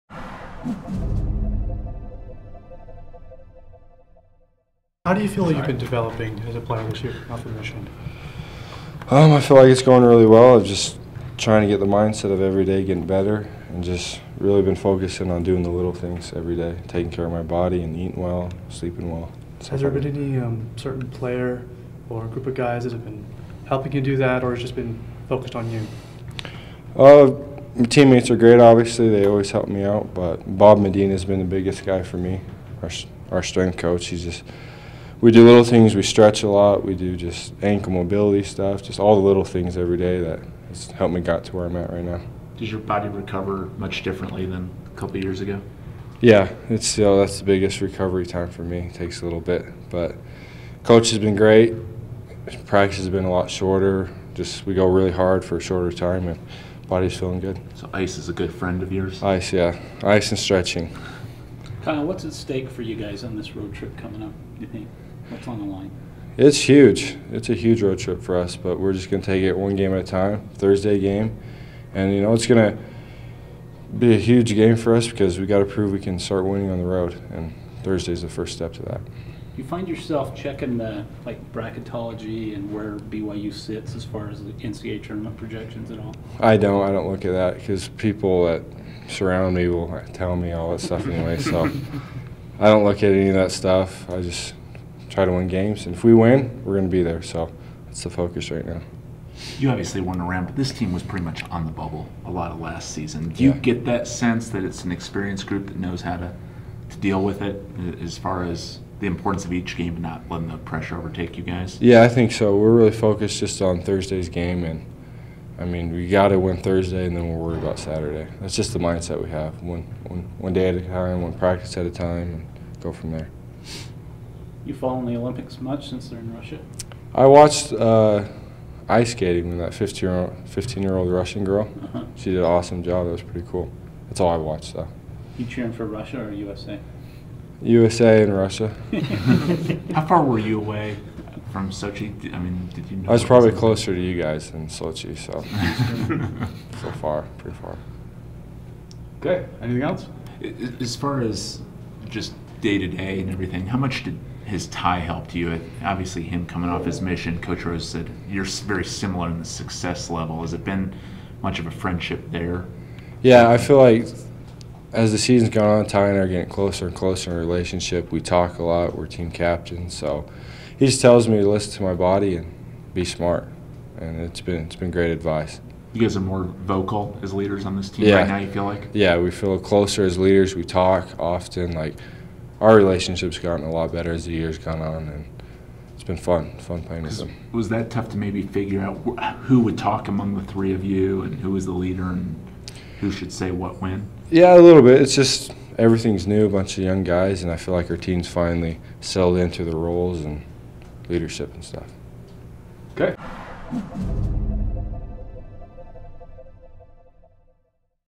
Kyle Collinsworth Post Practice 2-11-14
Kyle Collinsworth talks about adjusting to basketball post-mission and being a leader for this young squad. For more practice interviews and practice reports go to byutvsports.com